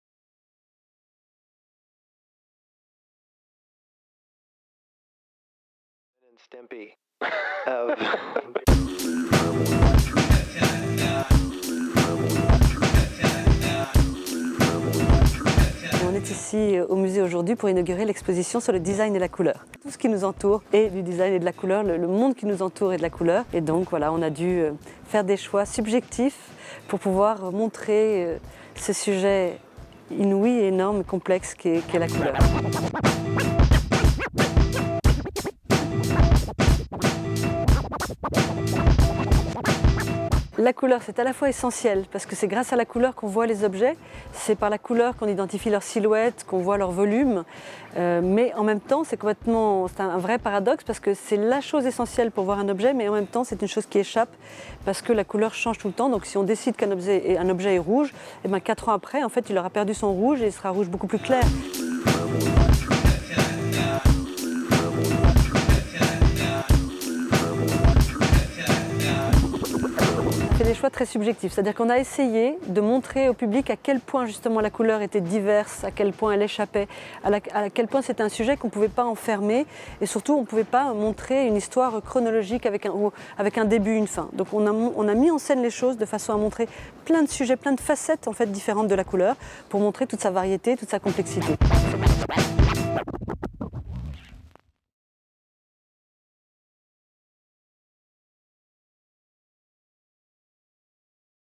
MADD / Oh couleurs !
Rencontre avec Constance Rubini, directrice du MADD Musée des Arts Décoratifs et du Design, autour de l'exposition « Oh couleurs ! Le design au prisme de la couleur », du 29 juin au 05 novembre 2017 au MADD à Bordeaux.